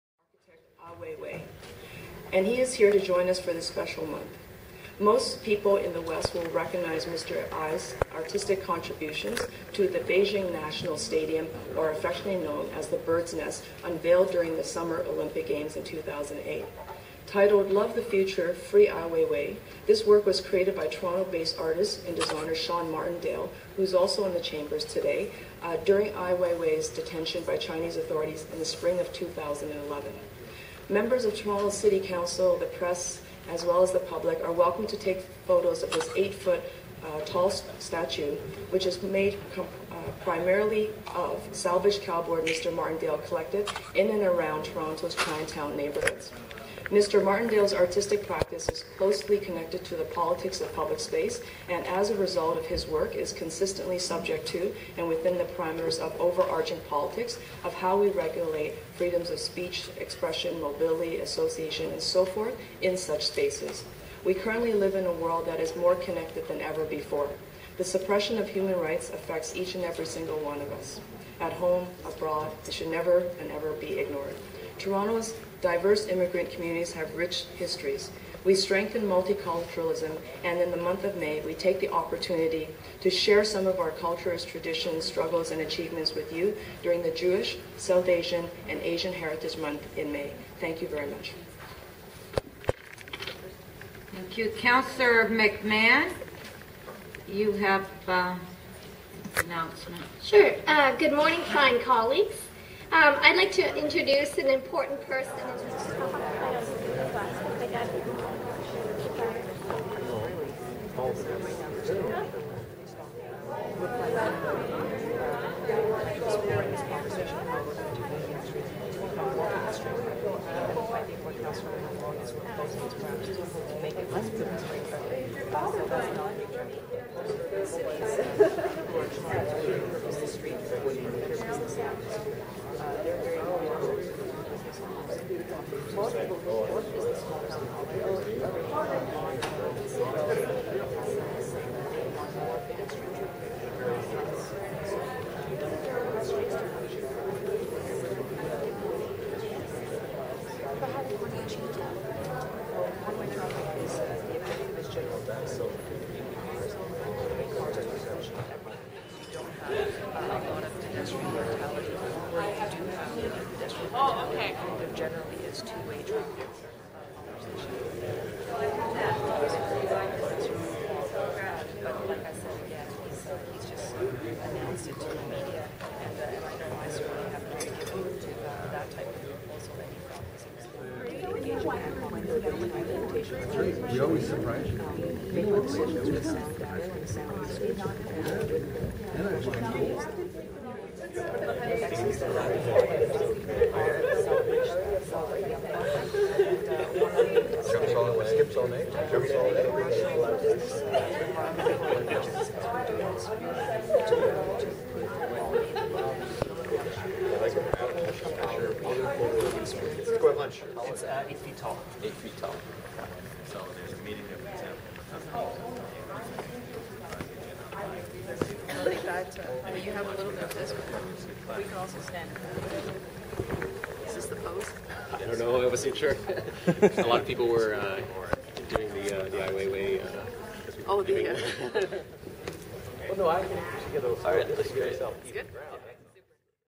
HiMY SYeD -- Ai WeiWei Cardboard Sculpture by Artist Sean Martindale, Toronto Council Chamber 5/8/12
HiMY SYeD -- Ai WeiWei Cardboard Sculpture by Artist Sean Martindale, Toronto Council Chamber May 8 2012